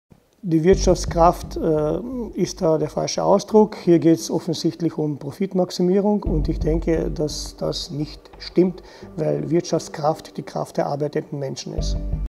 12 nrw17 | Mirko Messner (KPÖ+ Spitzenkandidat) | Flexible Arbeitszeiten
Mirko Messner (KPÖ+ Spitzenkandidat) im neuwal Barometer zur Nationalratswahl 2017 nrw17. 26 Positionen zur politischen Lage.